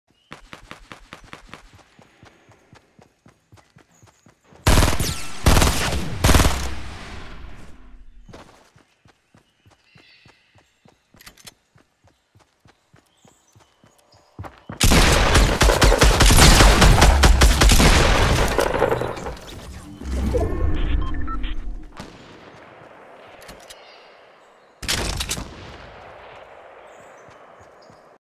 Looting Little Bandit
Thanks for stealing my purple scar you little POS that followed me around the entire game and did NOTHING to help.